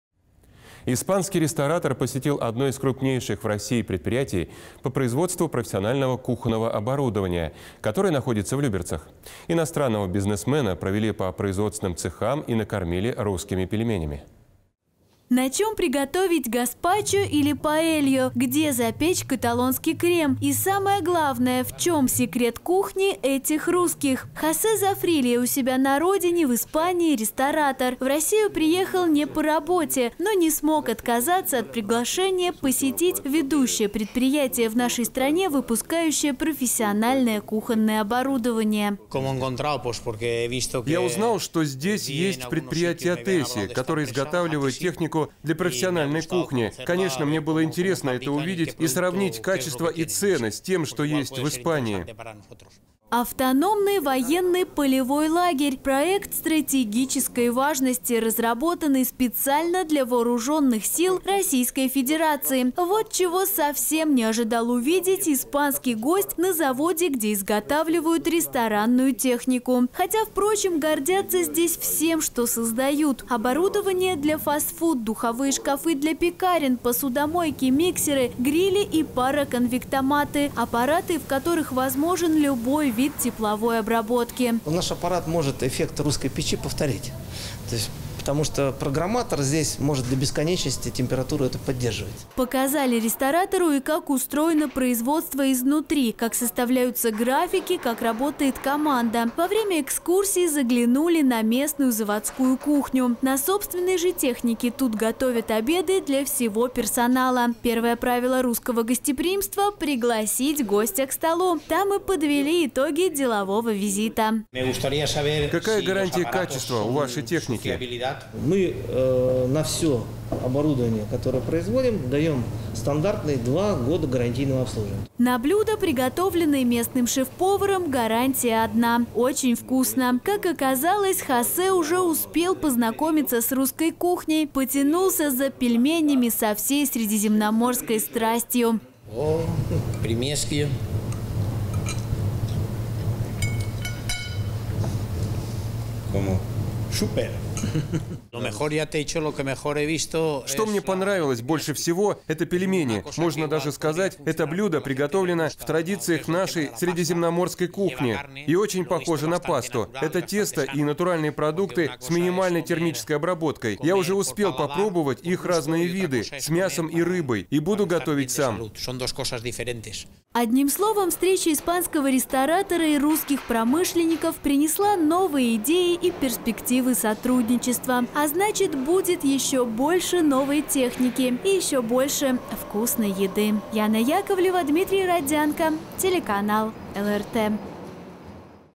Испанский ресторатор посетил люберецкое предприятие
Испанский ресторатор посетил одно из крупнейших в России предприятий по производству профессионального кухонного оборудования. Находится оно в Люберцах. Иностранного бизнесмена провели по производственным цехам и накормили русскими пельменями.

На чем приготовить гаспачо или паэлью? Где запечь каталонский крем? И самое главное – в чем секрет кухни этих русских? Хосе Зафрилья у себя на родине в Испании - ресторатор. В Россию приехал не по работе. Но не смог отказаться от приглашения посетить ведущее предприятие в нашей стране, выпускающее профессиональное кухонное оборудование.

Хосе Зафрилья, испанский ресторатор:

«Я узнал, что здесь есть предприятие «АТЕСИ», которое изготавливает технику для профессиональной кухни. Конечно, мне было интересно это увидеть и сравнить качество и цены с тем, что есть в Испании».

Автономный военный полевой лагерь – проект стратегической важности, разработанный специально для вооружённых сил Российской Федерации. Вот чего совсем не ожидал увидеть испанский гость на заводе, где изготавливают ресторанную технику. Хотя в прочем гордятся здесь всем, что создают. Оборудование для фаст-фуд, духовые шкафы для пекарен, посудомойки, миксеры грили и параконвектоматы – аппараты, в которых возможен любой вид тепловой обработки.

Сергей Шатилов, президент компании ООО «ПКО «АТЕСИ»:

«Наш аппарат может эффект русской печи повторить. Потому что программатор здесь может до бесконечности температуру эту поддерживать».

Показали ресторатору и как устроено производство изнутри: как составляются графики, как работает команда. Во время экскурсии заглянули на местную заводскую кухню. На собственной же технике тут готовят обеды для всего персонала. Первое правило русского гостеприимства – пригласить гостя к столу. Там и подвели итоги делового визита.

На блюда, приготовленные местным шеф-поваром гарантия одна – очень вкусно! Как оказалось, Хосе уже успел познакомиться с русской кухней. Потянулся за пельменями со всей средиземноморской страстью.

«Что мне понравилось больше всего - это пельмешки. Можно даже сказать, это блюдо приготовлено в традициях нашей, средиземноморской кухни. И очень похоже на пасту. Это тесто и натуральные продукты с минимальной термической обработкой. Я уже успел попробовать разные их виды: с мясом, с рыбой. И буду готовить сам».

Одним словом, встреча испанского ресторатора и русских промышленников принесла новые идеи и перспективы сотрудничества. А значит, будет еще больше новой техники. И еще больше вкусной еды.

Яна Яковлева, Дмитрий Родзянко